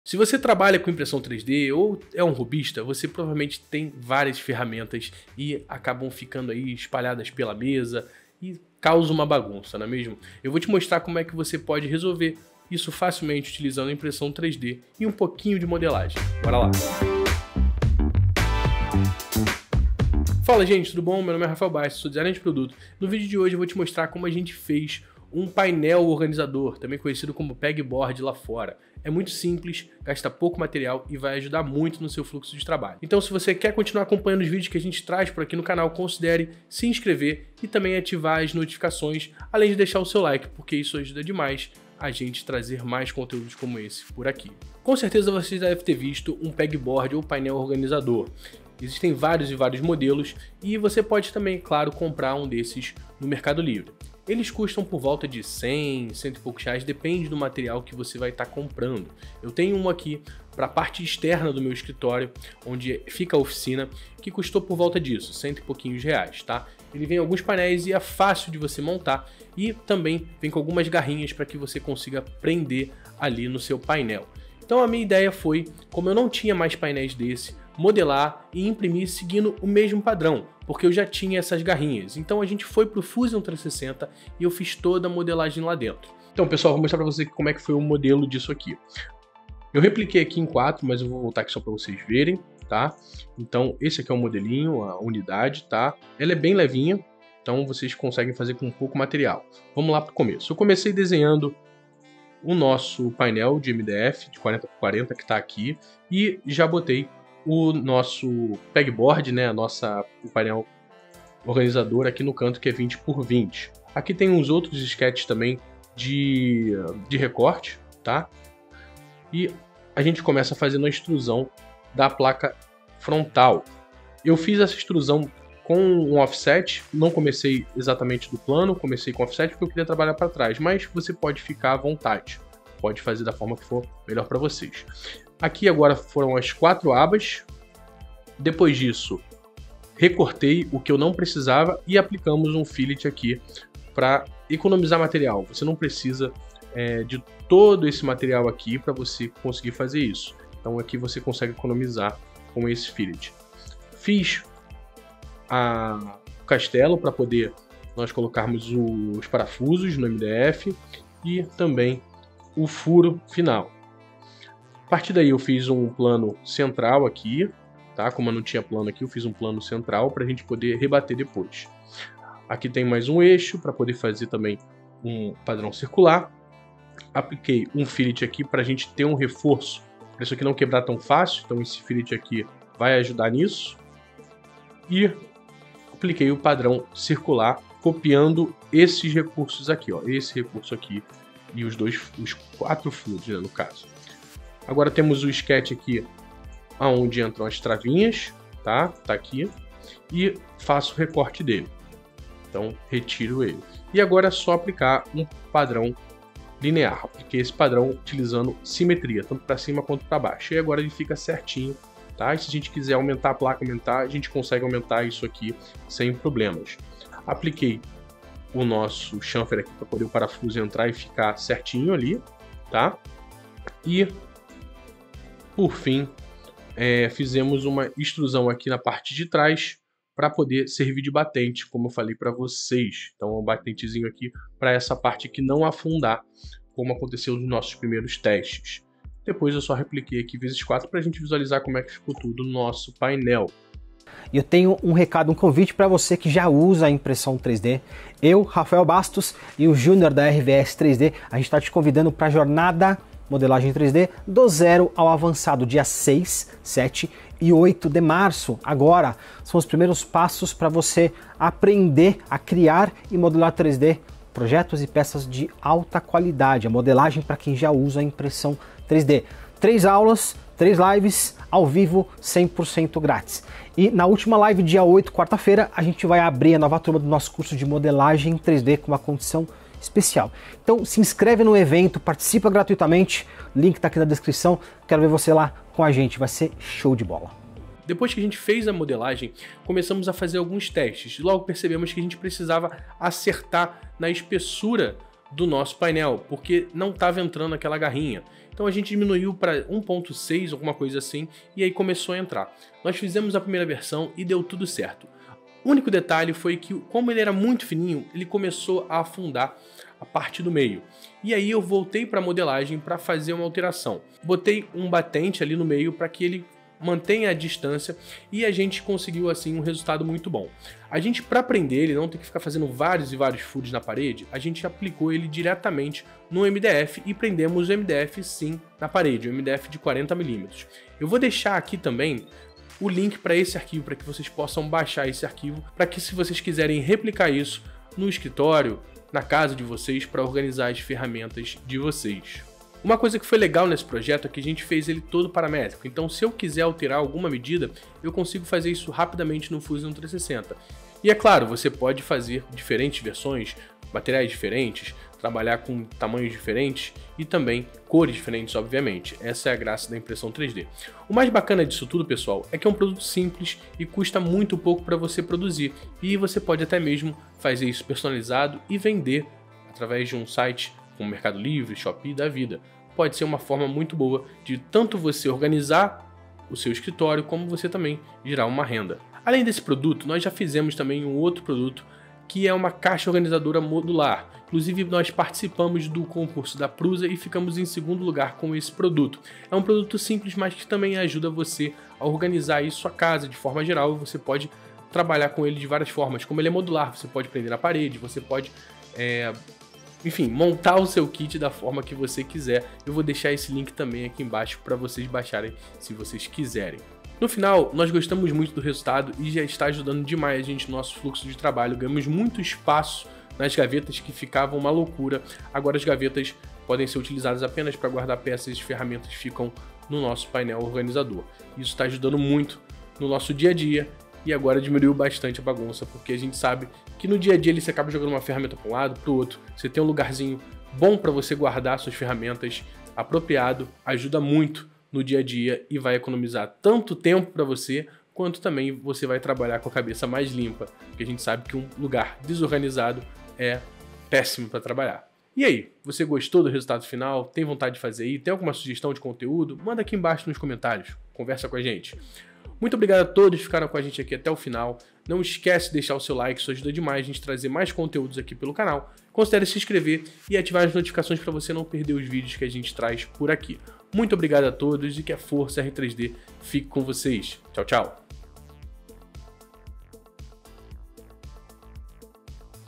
Impressão 3D e Organização
Vem ver um pouco como modelar, imprimir e fazer um PegBoard! Perfeito para organizar suas ferramentas para impressão 3D!

Para mais conteúdos como esse me siga no Instagram: @r3dsign ou meu no meu